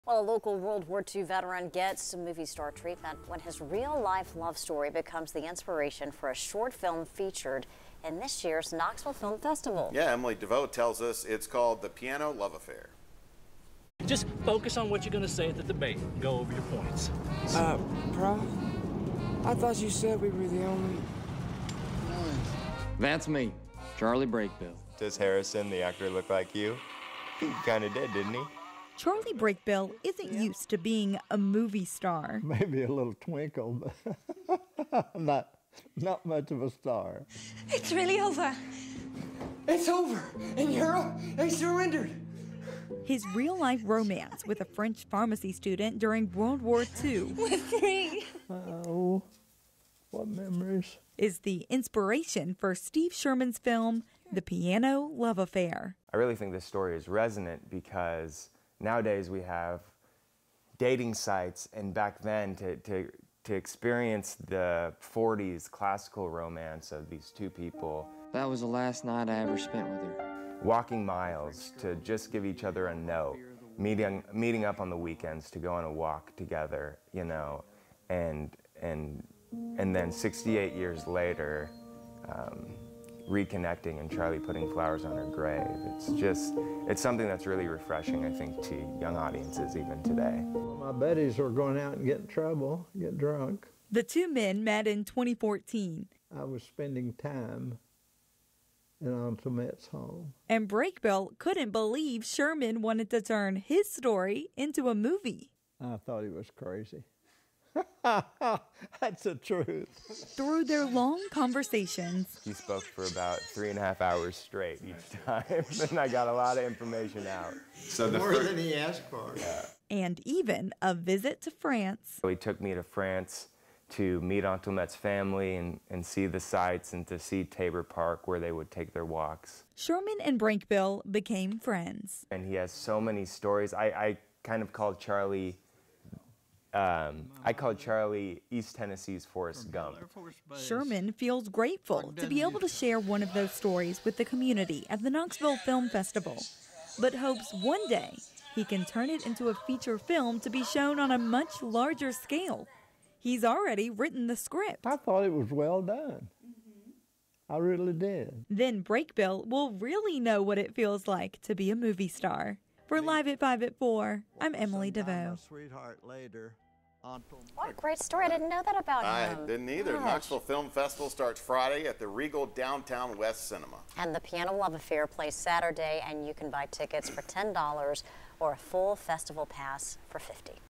Tennessee veteran's WWII romance becomes the inspiration for a new short film
68 years after falling in love with a French pharmacy student, Charlie Brakebill finally got to say goodbye.